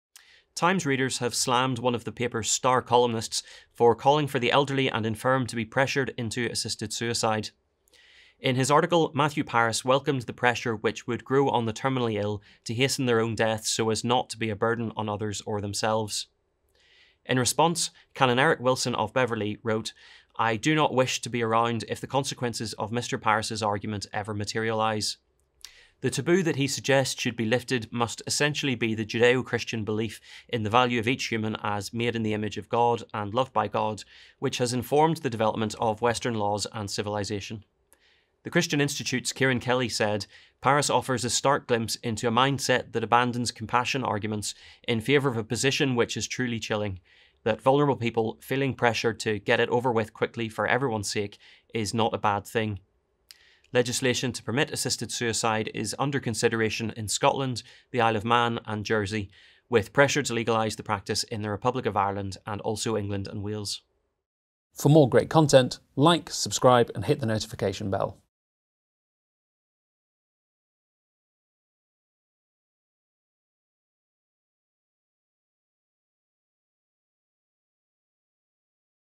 Columnist rebuked over assisted suicide
Times readers have slammed one of the paper’s star columnists for calling for the elderly and infirm to be pressured into assisted suicide.